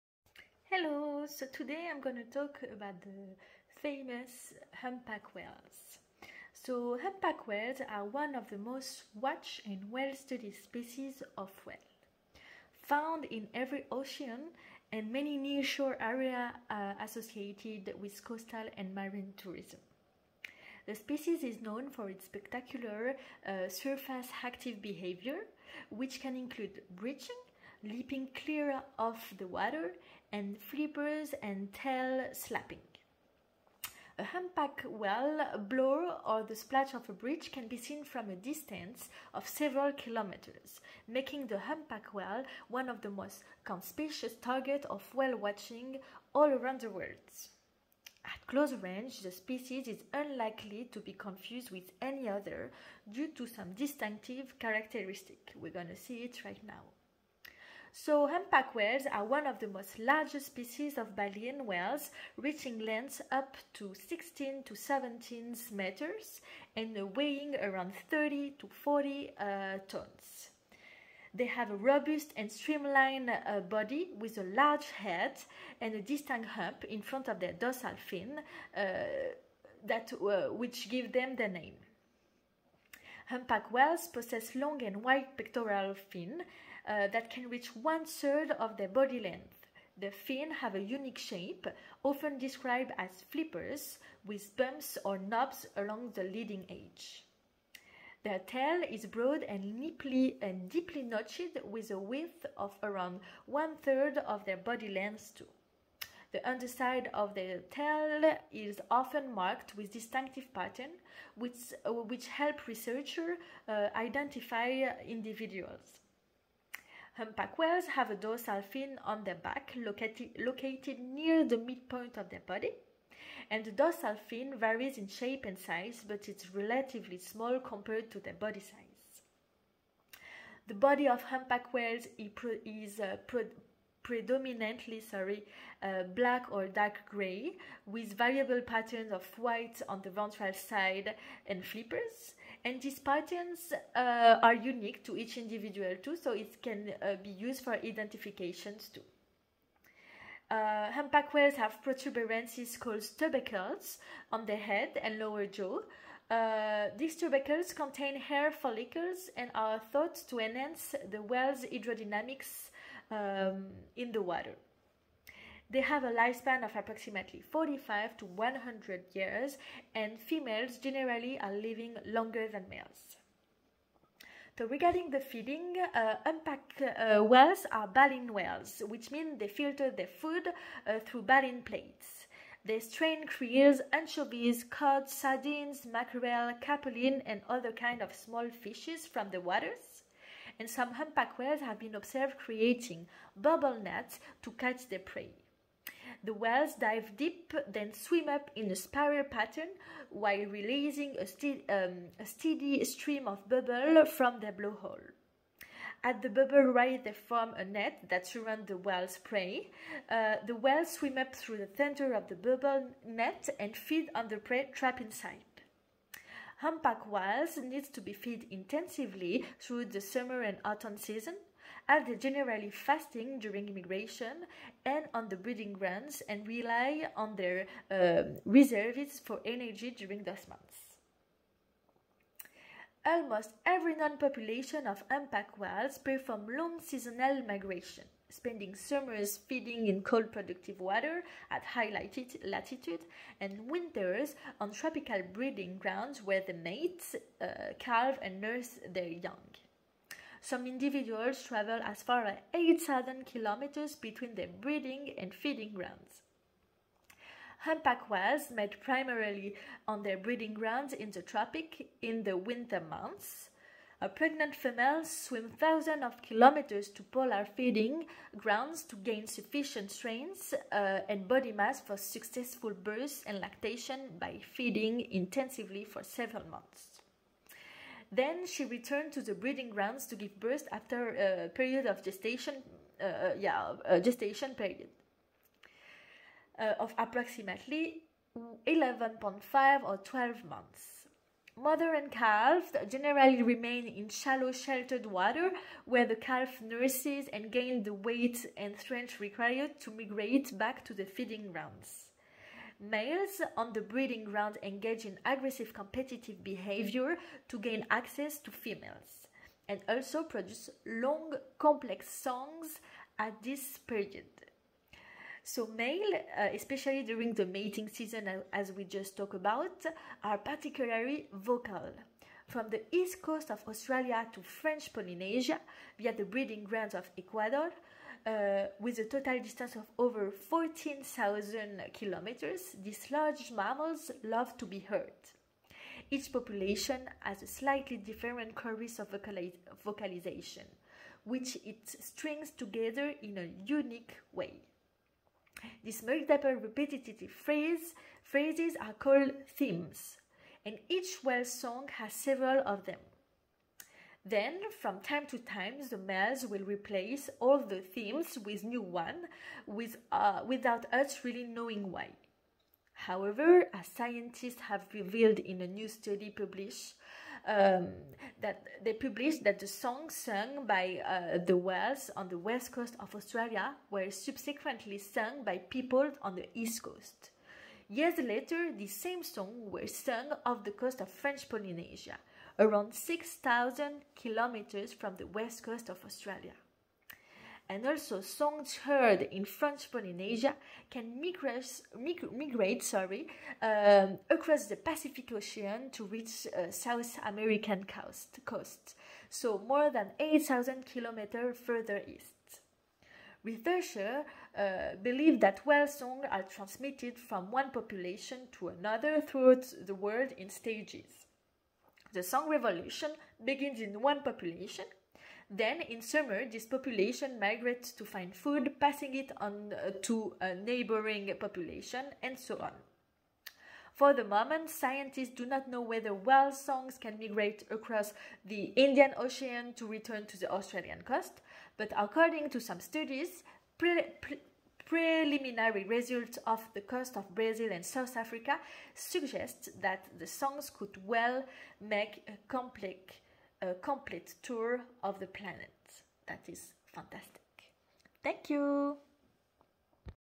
Gaelle's Humpback Whale Presentation
Check out Gaëlle's awesome Humpback Whale Presentation which she prepared for her Marine Guide Training course.

Congratulation Gaëlle on passing your Level 2 Marine Guide Training!